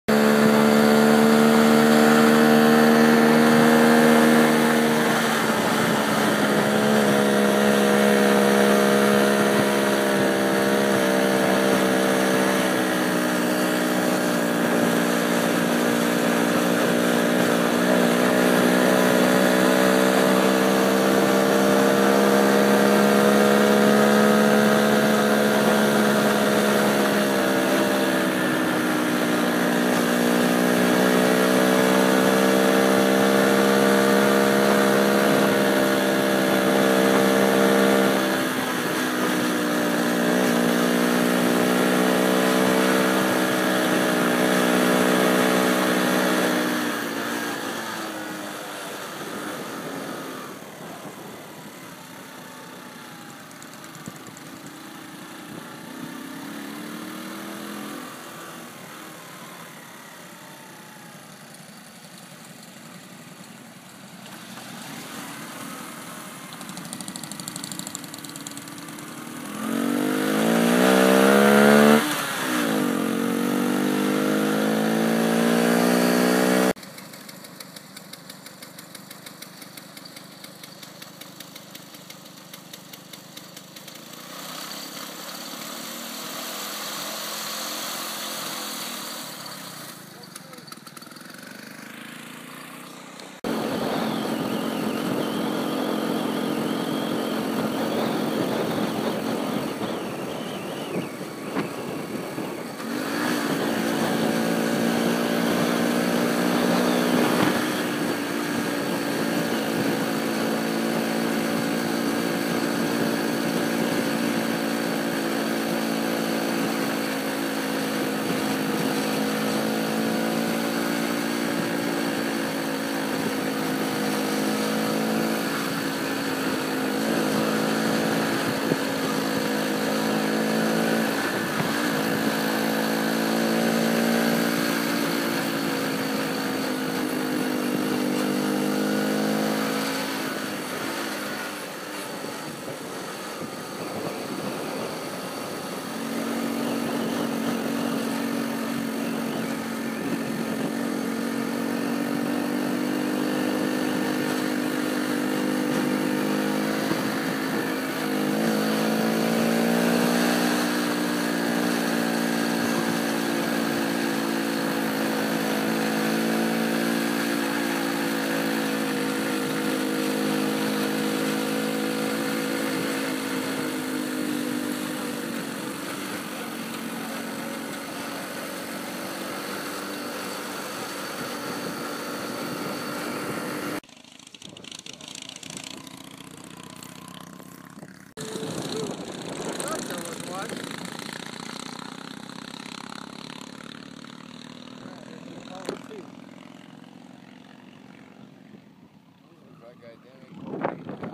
2014 Motorcycle Cannonball Endurance Run from Yakima
I had a fun morning on my wife's 1980 Honda C70 riding along with 2014 Motorcycle Cannonball Endurance Run from Yakima out to the Y at Hwy 12